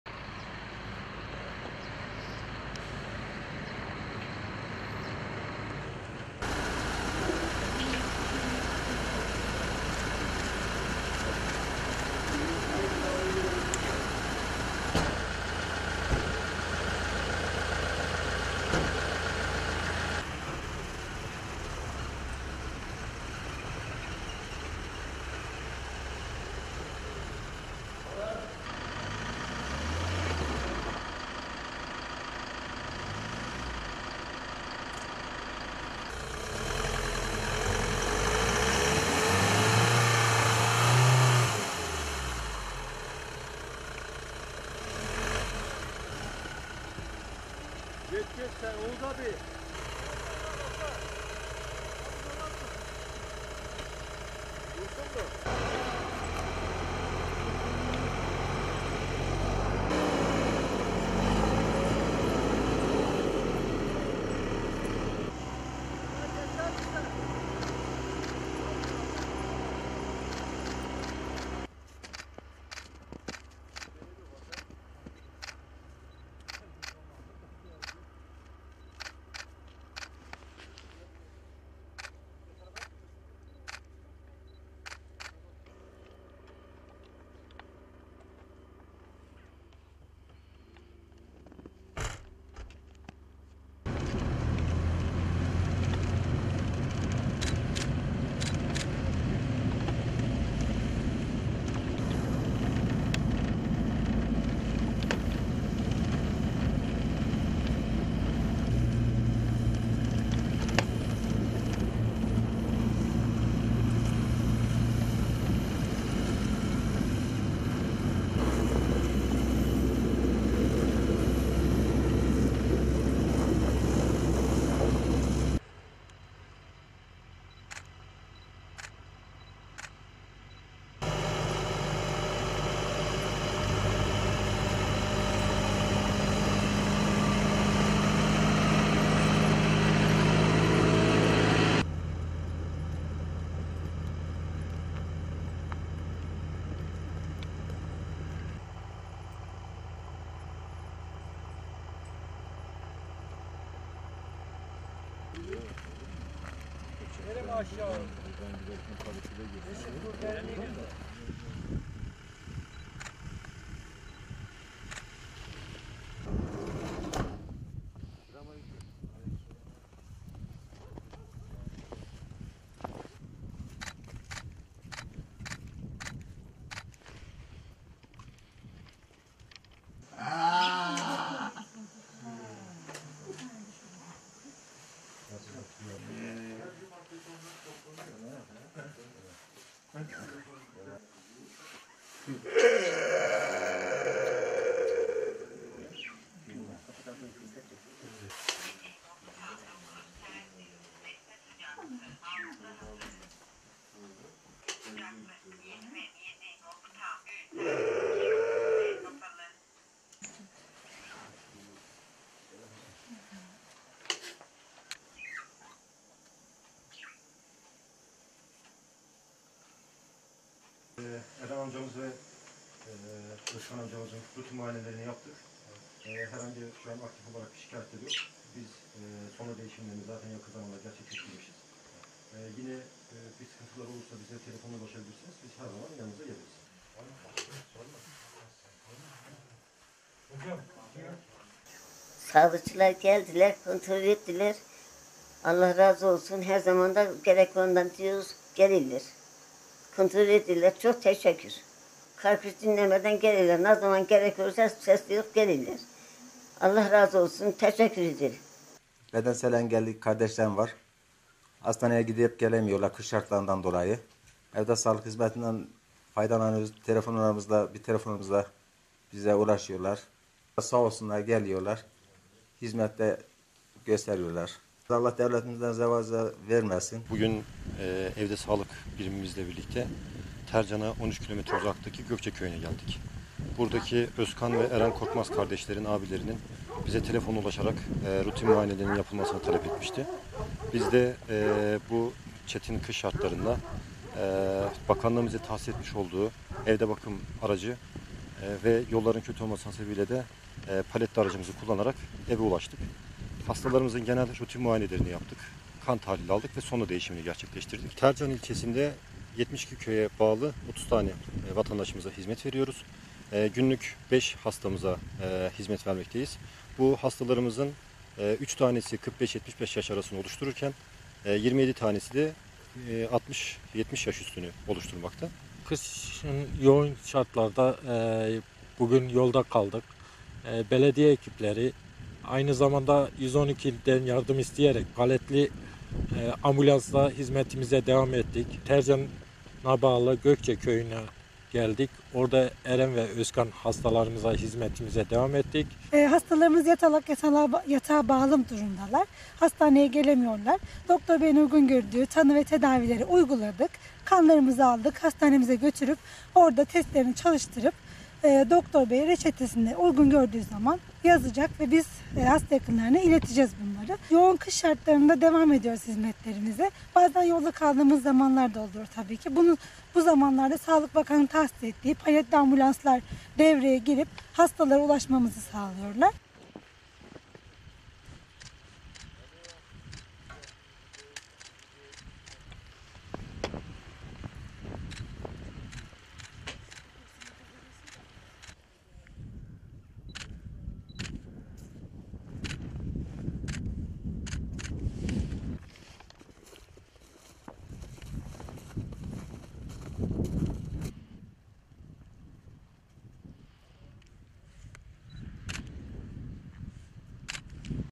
Karlı Yolları Aşan Evde Sağlık Hizmetleri Ekipleri Hastaların Umudu Oluyor
Erzincan İl Sağlık Müdürlüğüne bağlı Evde Sağlık Hizmetleri ekibi karlı yolları aşarak eve bağlı hastaların adeta umudu oluyor.

Evde sağlık hizmetleri ekipleri, çetin geçen kış aylarının zorlu koşullarına rağmen hastaların ayağına giderek rutin kontrollerini gerçekleştiriyorlar. Erzincan’a bağlı Tercan Devlet Hastanesi Evde Sağlık Hizmetleri ekibi, evde sağlık hizmeti almaya muhtaç hastaların kontrollerini ve tedavilerini yapabilmek için kar, kış demeden sağlık hizmeti götürmeye devam ediyor. Zaman zaman kar ve tipi nedeniyle ilerleyemeyerek yolda kalan Evde Sağlık Hizmetleri aracının imdadına ise paletli ambulans yetişiyor. Tam donanımlı paletli ambulanslarla yüksek rakımlı köylere ulaşan Evde Sağlık Hizmetleri ekipleri çalışmalarını aksatmadan vatandaşların umudu olmaya devam ediyor.

Ekipler, kış şartları nedeniyle hastaneye gidemeyecek durumda olan engelli 2 hastanın kardeşlerinin araması üzerine kontrollerini yapmak için Tercan ilçesine 13 kilometre uzaklıkta bulunan Gökçe köyüne gelerek hastaların tedavilerini gerçekleştirerek rutin kontrollerini yaptılar.

'Bir telefonla çıkıp geliyorlar'
Evde sağlık hizmetlerine teşekkür eden hasta yakını Nevriye Korkmaz, "Sağlıkçılar geldiler, kontrollerini yaptılar. Her zaman da geliyorlar. Ne zaman telefon açsak kar-kış dinlemeden çıkıp geliyorlar. Allah hepsinden razı olsun. Teşekkür ediyorum" dedi.

Evde sağlık birimince kontrolleri evinde yapılan ve bedensel engelleri yüzünden kış şartlarında hastaneye gidemeyen Özkan ve Eren Korkmaz’ın kardeşi Erkan Korkmaz da konuşmasında Evde Sağlık Hizmetlerinin bir telefonla çıkıp geldiklerinin altını çizerek, "Bedensel engelli kardeşlerim var. Kış şartlarından dolayı hastaneye gidip gelemiyorlar. Evde sağlık hizmetleri bir telefonumuz ile bize ulaşıyorlar. Kendilerine teşekkür ediyoruz. Sağ olsunlar gelip hizmet ediyorlar. Allah devletimize zeval vermesin" diye konuştu.

Tercan Devlet Hastanesi Başhekimi Dr. Murat Kaya, zorlu kış şatlarına aldırmadan çalışmalarına devam ettiklerini belirterek, "Evde sağlık birimimiz ile birlikte Tercan'a 13 kilometre uzaklıkta bulunan Gökçe köyüne geldik. Burada ki Özkan ve Eren Korkmaz isimli hastaların kardeşi bize telefonla ulaşarak rutin tedavilerinin yapılmasını talep etmişti. Biz de bu çetin kış şartlarında bakanlığın bize tahsis etmiş olduğu evde bakım aracı ve yolların kötü olması hasebiyle de paletli aracımızı kullanarak eve ulaştık. Hastalarımızın genel rutin muayenelerini yaptık. Kan tahlili aldık ve sonda değişimlerini gerçekleştirdik. Evde sağlık birimi olarak herhangi bir sebepten dolayı yatağa bağımlı ya da evde sağlık hizmeti alması gereken hastalarımızın psikolojik ve sosyal danışmanlığını içinde kapsayacak şekilde tahlillerini, tedavilerini ve rutin muayenelerini gerçekleştiriyoruz. Tercan ilçesine 72 köye bağlı 30 vatandaşımıza hizmet veriyoruz. Günlük 5 hastaya hizmet vermekteyiz. Bu hastaların 3 tanesi 45-75 yaş arasını oluştururken, 27 tanesi de 60-70 yaş üstünü oluşturmakta" ifadelerini kullandı.

Evde Sağlık Hizmetleri biriminde görevli Yasin Yıldırım ise yolda kaldıklarında paletli ambulans ile yola devam ettiklerini söyleyerek, "Yoğun kış şartlarında bugün yolda kaldık. Belediye ekiplerinden ve 112'den yardım istedik. Paletli ambulansla yolumuza devam ettik. Tercan'a bağlı Gökçe köyüne geldik ve hastalarımıza hizmet etmeye devam ettik" ifadelerine yer verdi.

Hemşire Yeşim Güler de konuşmasında, "Hastalarımız yatalak, yatağa bağlı durumdalar ve hastaneye gelemiyorlar. Doktor beyin uygun gördüğü tanı ve tedavileri uyguladık. Kanlarımızı aldık, hastaneye götürüp testlerini çalıştırıp doktor beye ileteceğiz. Reçetesi yazılacak ve biz de hasta yakınlarına ileteceğiz. Hizmetlerimize yoğun kış şartlarında devam ediyoruz. Bazen yolda kaldığımız zamanlar da oluyor tabii ki. Bu zamanlarda paletli ambulanslar devreye girip hastalara ulaşmamızı sağlıyorlar" dedi.